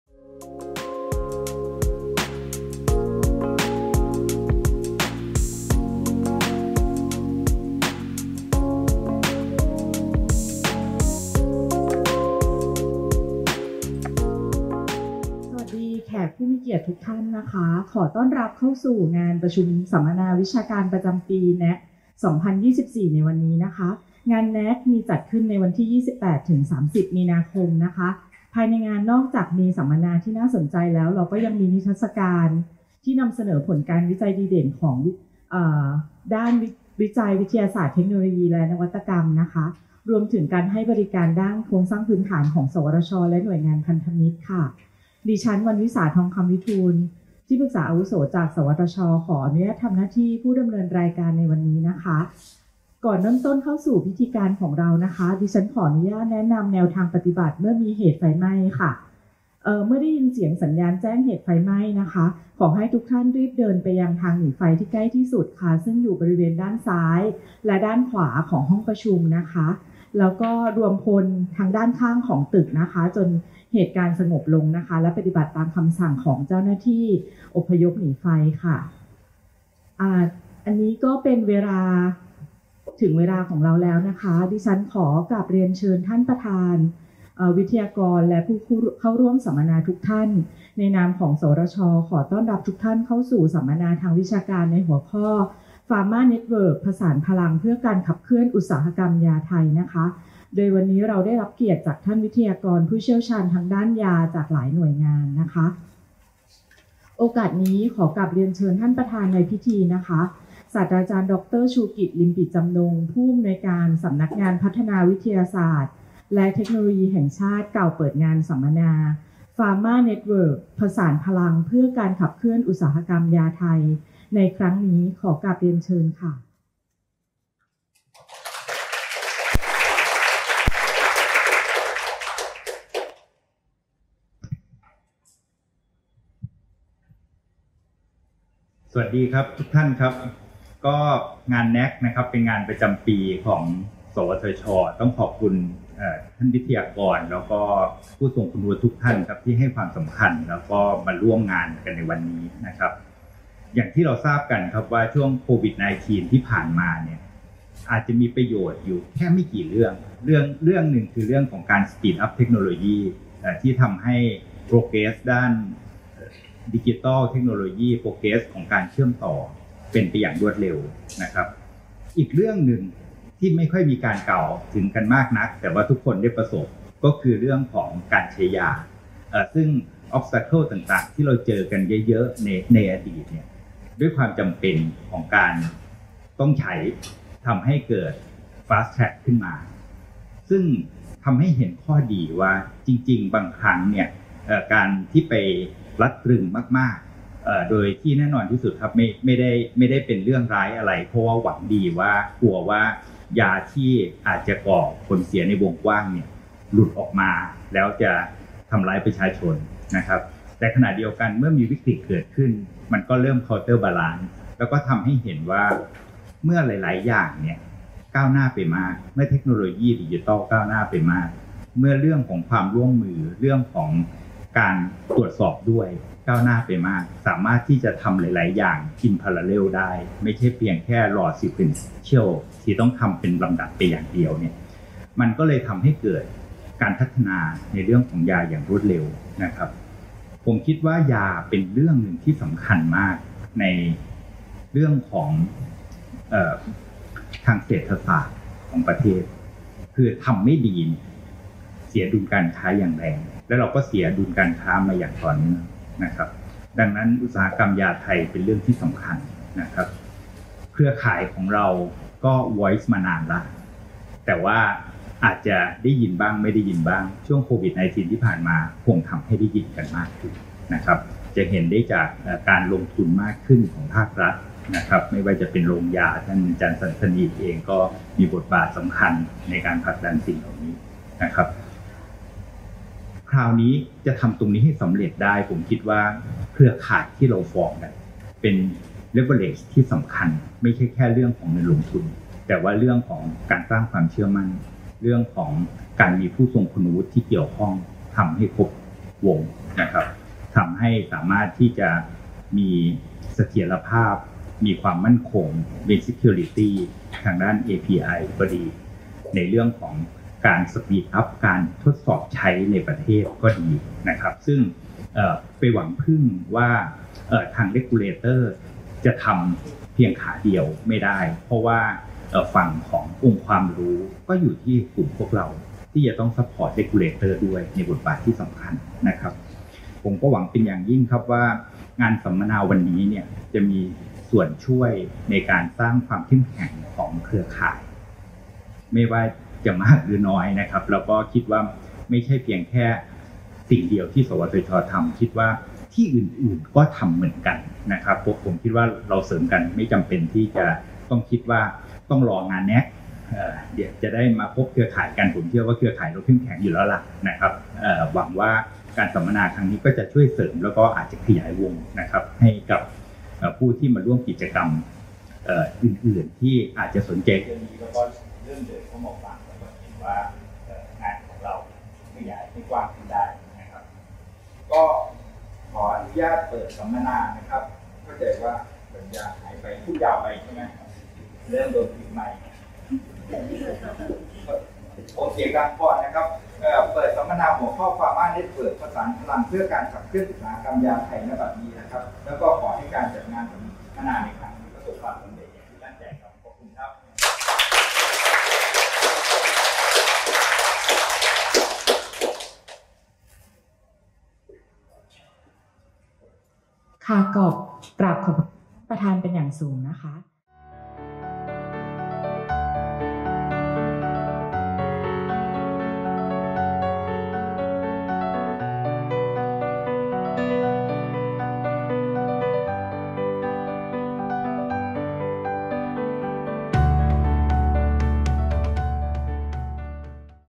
Pharma NETwork…ผสานพลังเพื่อการขับเคลื่อนอุตสาหกรรมยาไทย (1/5)
กล่าวเปิดการสัมมนา "Pharma NETwork…ผสานพลังเพื่อการขับเคลื่อนอุตสาหกรรมยาไทย (Pharma NETwork…Integrated Power for Driving the Thai Pharmaceutical Industry)" โดย ศ. ดร.ชูกิจ ลิมปิจำนงค์ ผู้อำนวยการ สำนักงานพัฒนาวิทยาศาสตร์และเทคโนโลยีแห่งชาติ วันที่ 28 มีนาคม 2567 เวลา 08.30 – 12.00 น. ณ ห้อง CC-405 อาคาร 14 (CC) อุทยานวิทยาศาสตร์ประเทศไทย จ.ปทุมธานี

การประชุมวิชาการประจำปี สวทช. ครั้งที่ 19 หรือ NAC2024 ภายใต้แนวคิด “สานพลัง สร้างงานวิจัย พลิกโฉมเศรษฐกิจและสังคมไทยด้วย BCG Implementation” ระหว่างวันที่ 28-30 มีนาคม 2567 ณ อุทยานวิทยาศาสตร์ประเทศไทย จ.ปทุมธานี